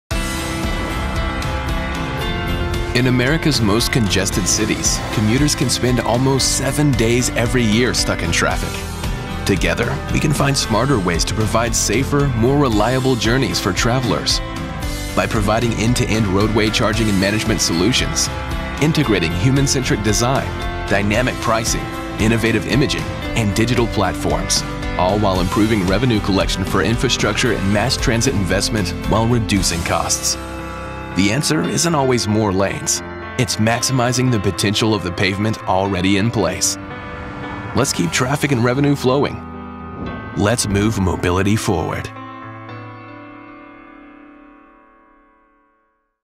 Conduent Transportation: Roadway Charging & Management Overview Video
With the latest technology-fueled tolling, road usage and congestion management solutions, Conduent Transportation can help you get more people where they’re going while improving revenue collection and reducing costs.